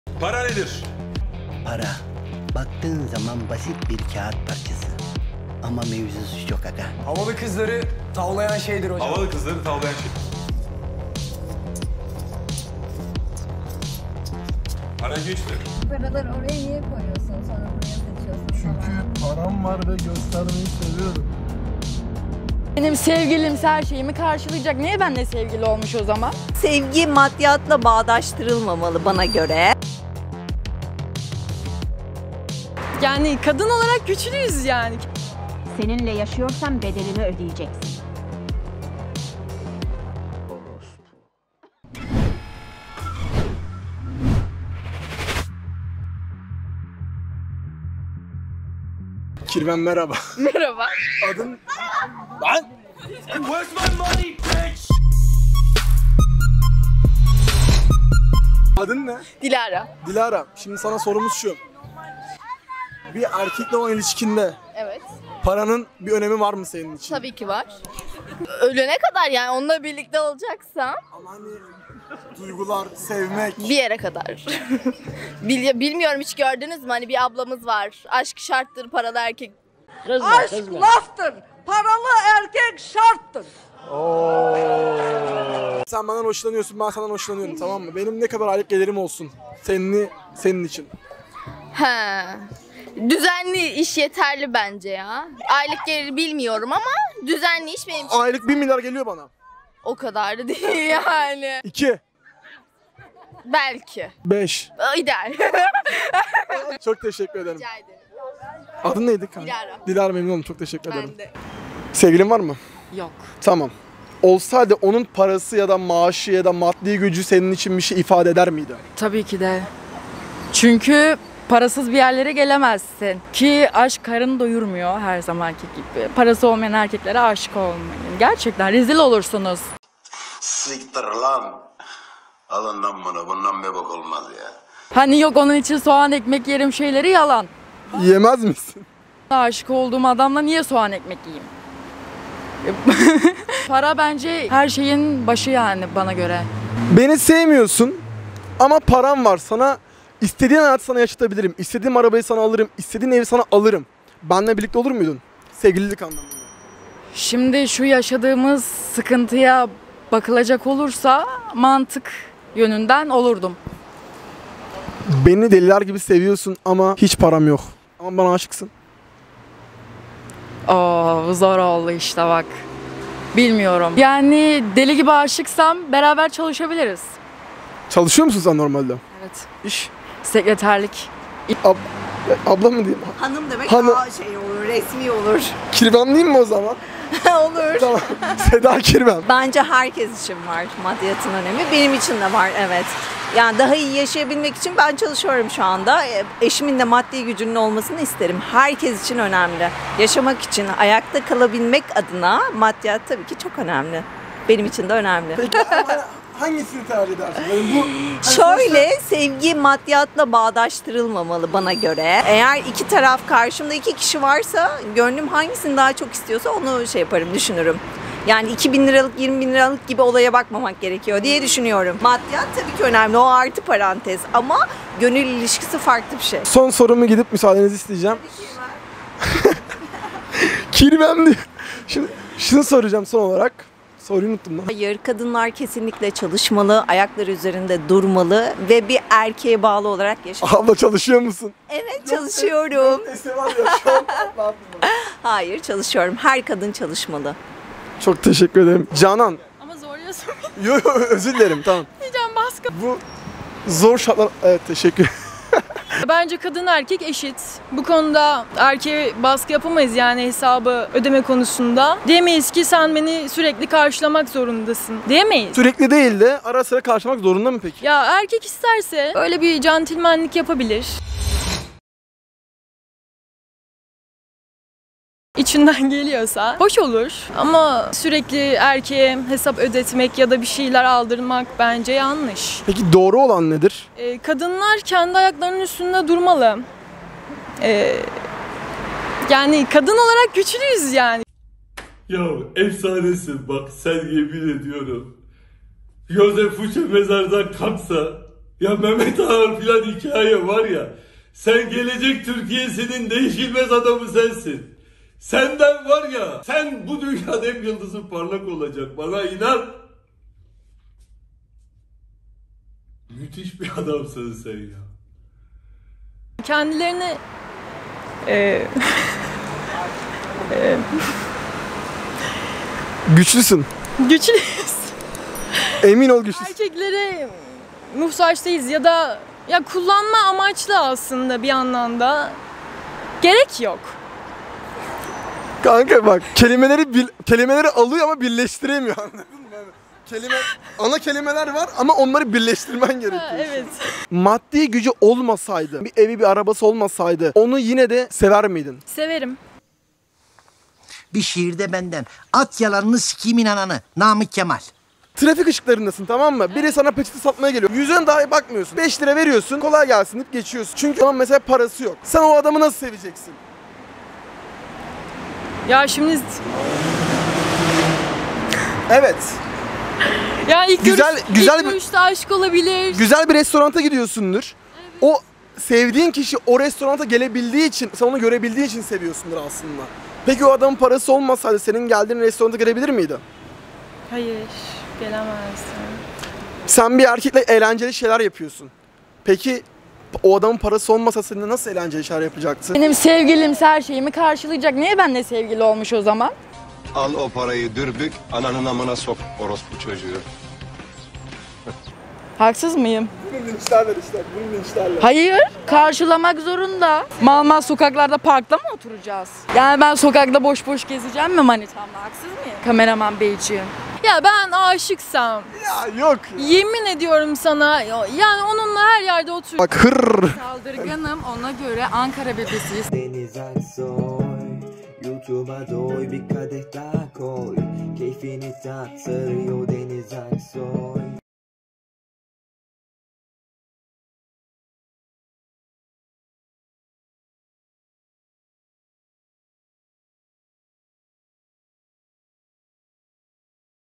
KIZLARA SORDUK ; Paranın Önemi #2 SOKAK RÖPORTAJI
Kirvelerim merhaba bugün sokak röportajları serisine bir video daha ekliyoruz. Biliyorsunuz bu videolarda sadece kızlar ile röportaj yapıyoruz, ileride bakacağız. Kız cinsiyetli kirvelerimize para / paranın önemi tarzı sorular sorduk; çok değişik cevaplar aldık. Umarım siz de beğenirsiniz.

Instagramım: denizaksoytv